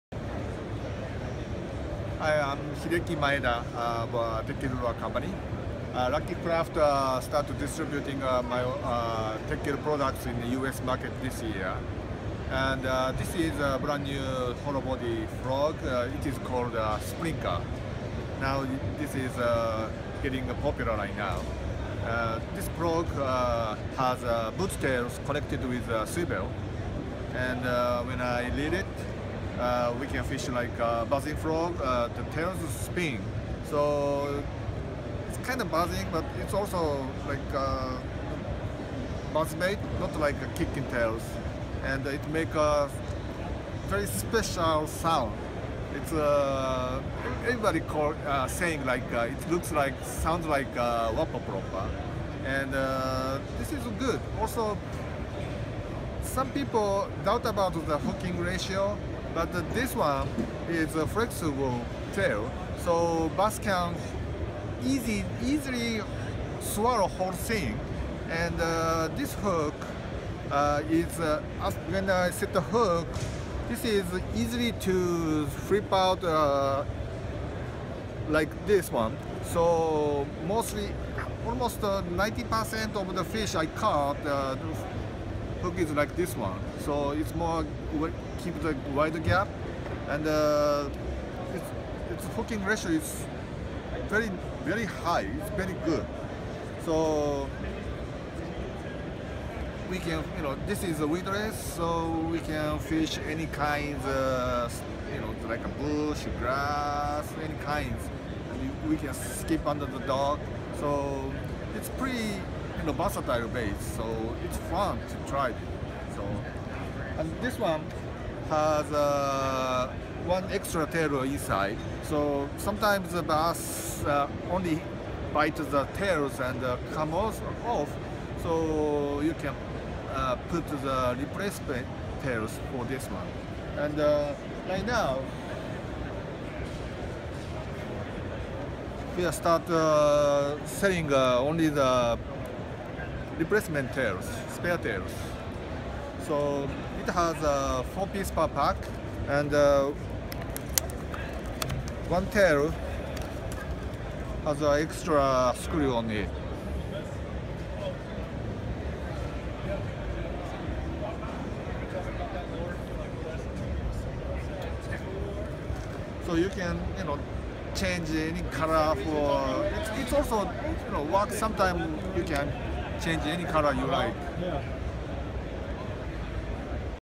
iCAST 2017 NEW Teckel Sprinkle Frog! - Hideki Maeda
I had the opportunity to go to Icast this year and check out some of the fishing companies out there! I met up with Hideki Maeda, creator of the Sprinkle frog! He has created, which I believe, is going to be the hottest lure out there in 2017. The owners of Lucky Craft has brought Hideki on board with them. Good luck finding these, they are very RARE.